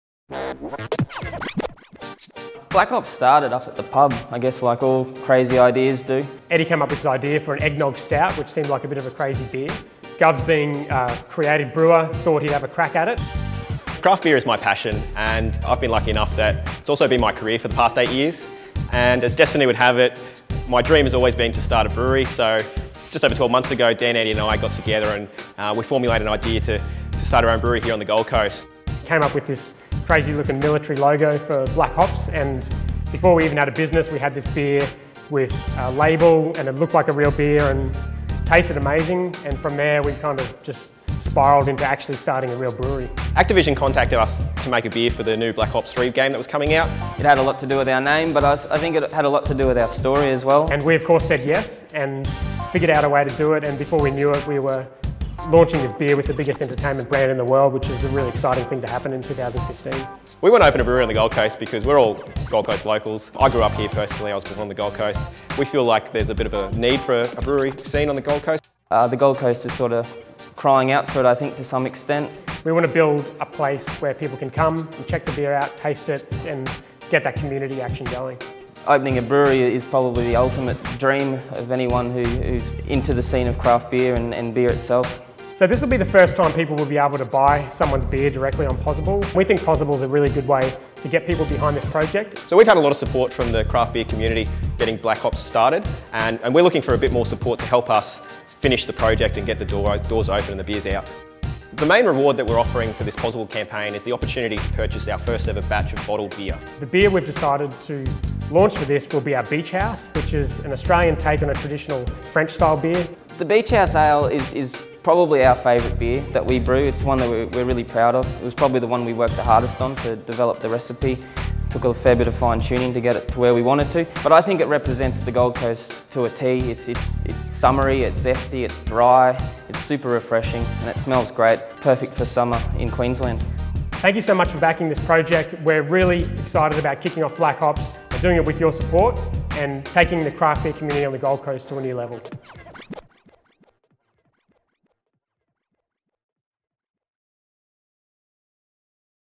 Black Hops - Australia's first brewery to launch via crowdfunding
We are aiming to become Australia's first brewery to successfully launch via crowdfunding. to get behind the project.